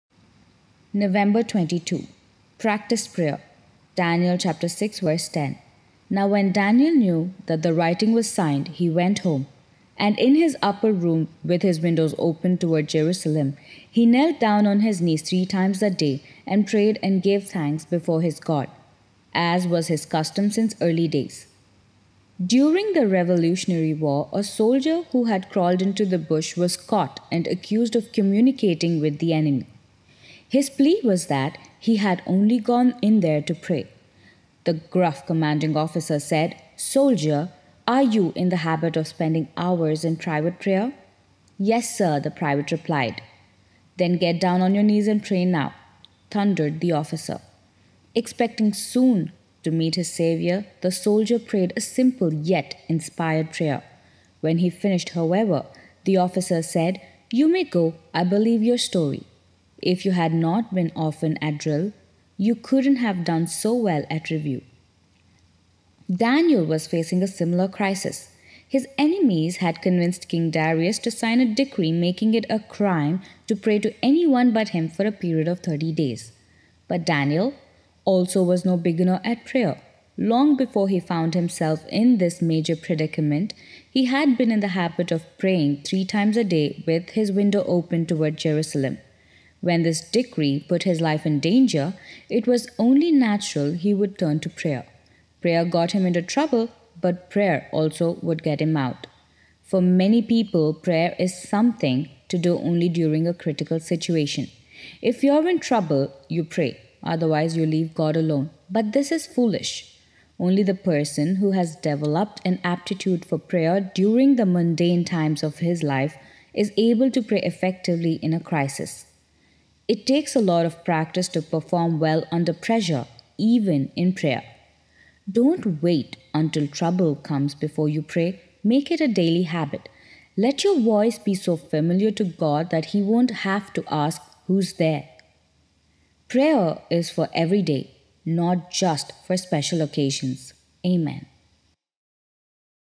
English | Nov 22 | Daniel | Giants of the OT | Woodrow Kroll
November-22 : Lessons on Living from DANIEL
PRACTICED PRAYER
DANIEL 6:10 : Now when Daniel knew that the writing was signed, he went home. And in his upper room, with his windows open toward Jerusalem, he knelt down on his knees three times that day, and prayed and gave thanks before his God, as was his custom since early days.
During the Revolutionary War a soldier who had crawled into the bush was caught and accused of communicating with the enemy.
His plea was that he had only gone in there to pray. The gruff commanding officer said, "Soldier, are you in the habit of spending hours in private prayer?" "Yes, sir," the private replied. "Then get down on your knees and pray now!" thundered the officer.
Expecting soon to meet his Savior, the soldier prayed a simple yet inspired prayer. When he finished, however, the officer said, "You may go I believe your story. If you had not been often at drill, you couldn't have done so well at review!"
Daniel was facing a similar crisis. His enemies had convinced King Darius to sign a decree making it a crime to pray to anyone but him for a period of 30 days.
But Daniel also was no beginner at prayer, Long before he found himself in this major predicament, he had been in the habit of praying three times a day with his windows open toward Jerusalem, When this decree put his life in danger, it was only natural he would turn to prayer. Prayer got him into trouble, but prayer also would get him out.
For many people, prayer is something to do only during a critical situation. If you're in trouble, you pray; otherwise you leave God alone. But this is foolish.
Only the person who has developed an aptitude for prayer during the mundane times of life is able to pray effectively in a crisis. It takes a lot of practice to perform well under pressure, even in prayer.
Don't wait until trouble comes before you pray, Make it a daily habit. Let your voice be so familiar to God that He won't have to ask, "Who's there?"
PRAYER IS FOR EVERY DAY, NOT JUST FOR SPECIAL OCCASIONS.

IFSC-CNRB0013313
Branch code- 13313
Current account